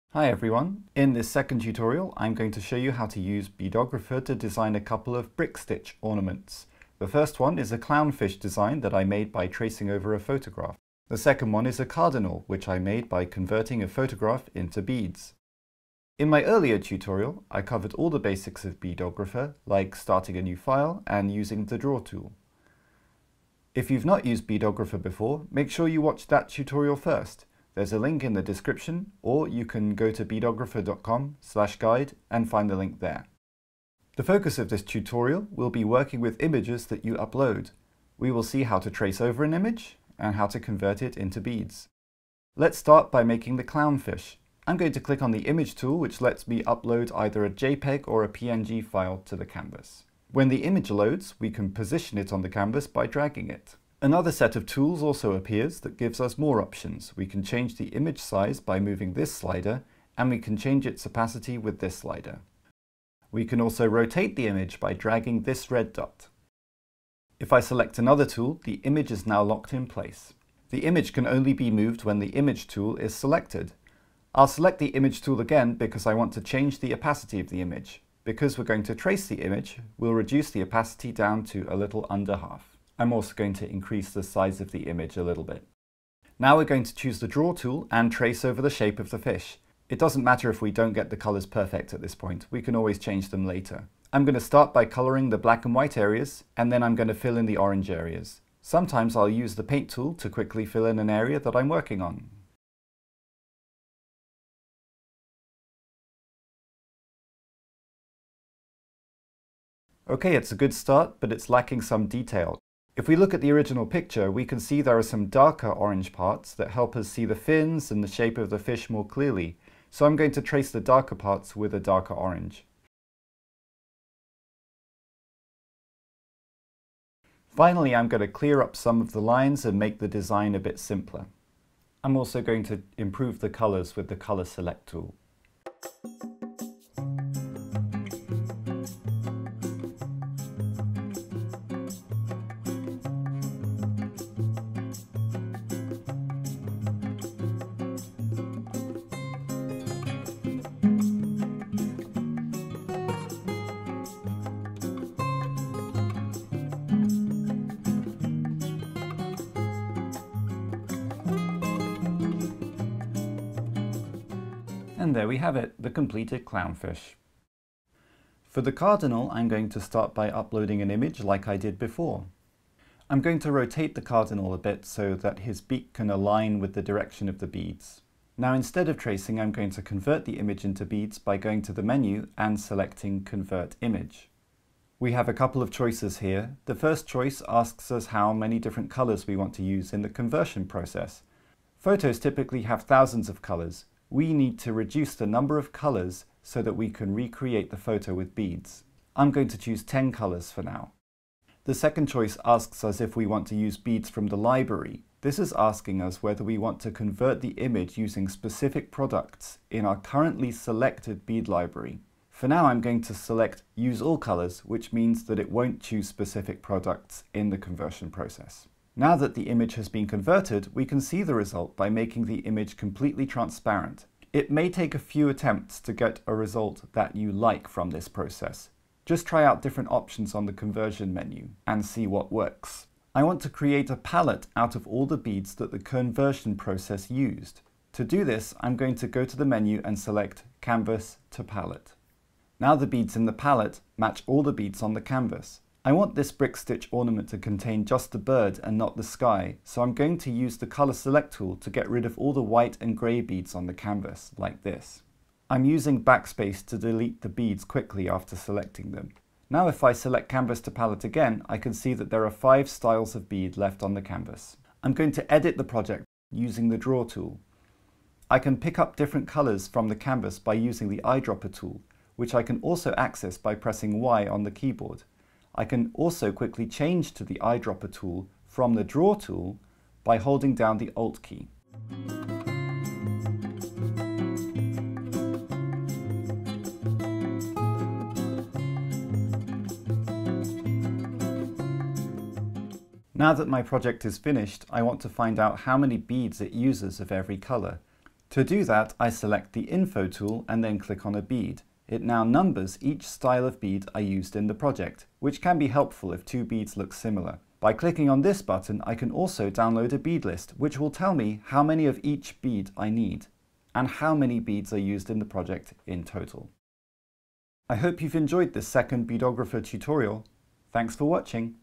ARCHIVED: Beadographer Tutorial Part 2
Beadographer is a web app that lets you design beading projects. Learn more and try out the tool at beadographer.com.